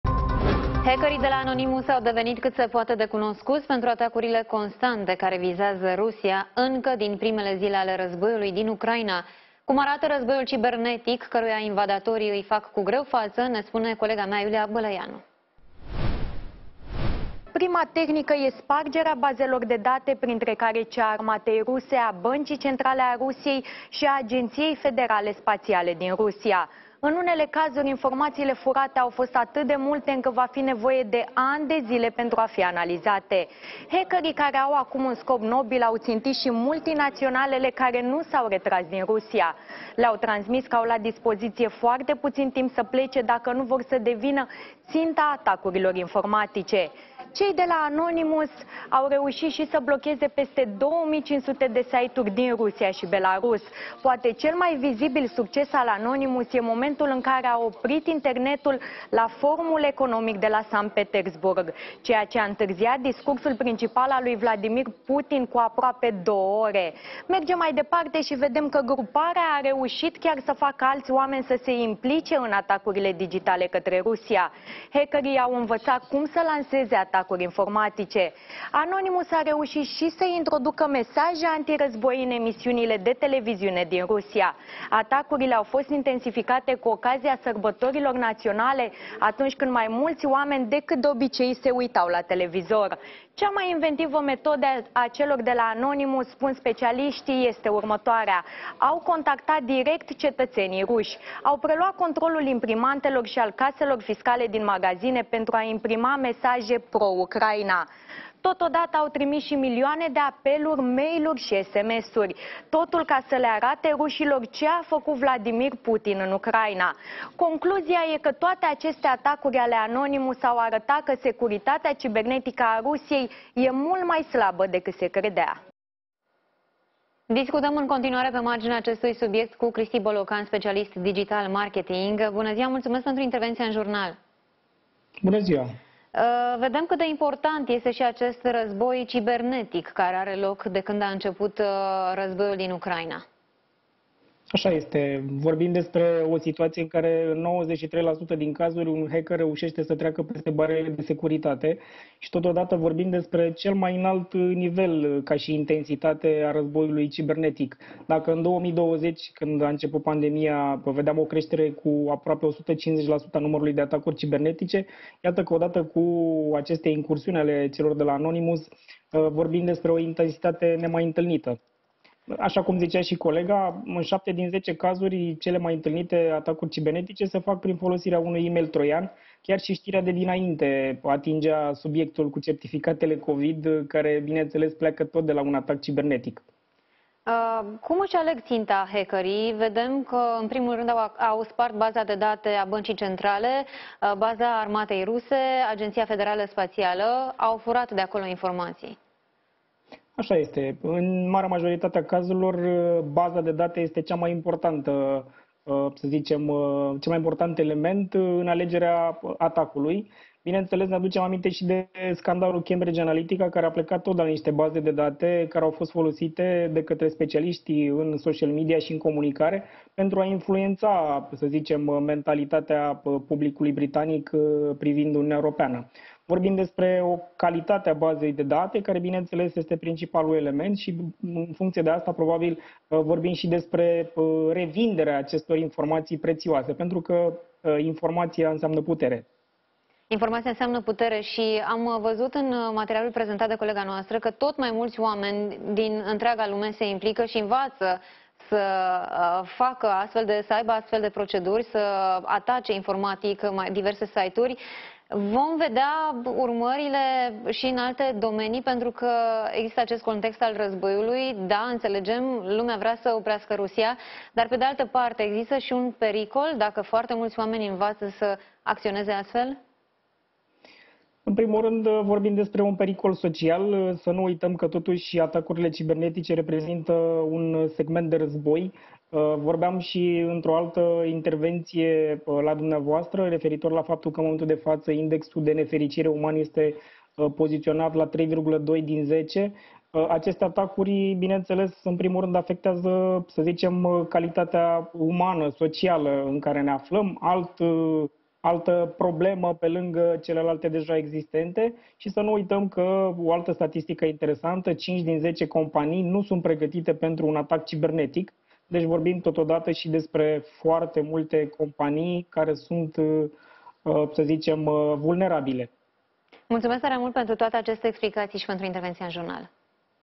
Hackerii atacă în mod constant Rusia. Cum arată războiul cibernetic
Hackerii de la Anonymous au devenit cât se poate de cunoscuți pentru atacurile constante care vizează Rusia încă din primele zile ale războiului din Ucraina.